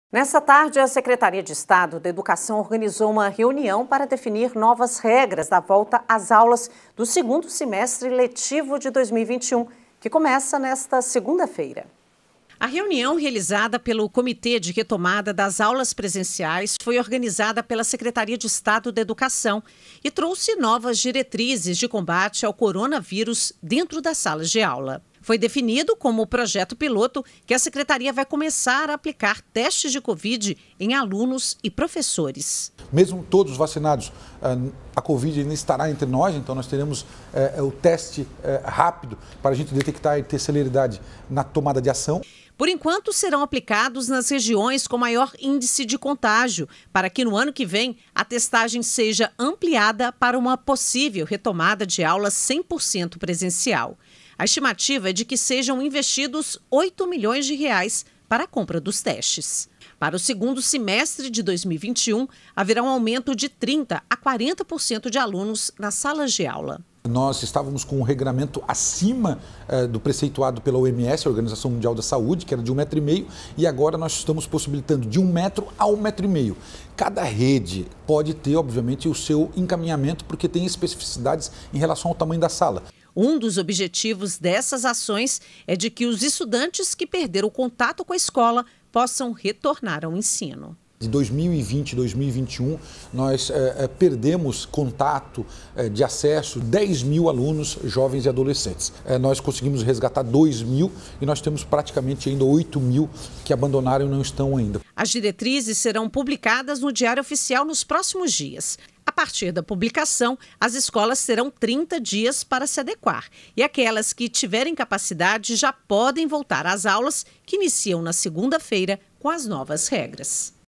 Secretaria de Estado da Educação define novas regras de volta às aulas
Na tarde desta quinta-feira (29), a Secretaria de Estado da Educação organizou uma reunião para definir novas regras da voltas às aulas do segundo semestre letivo de 2021, que começa nesta segunda-feira (2).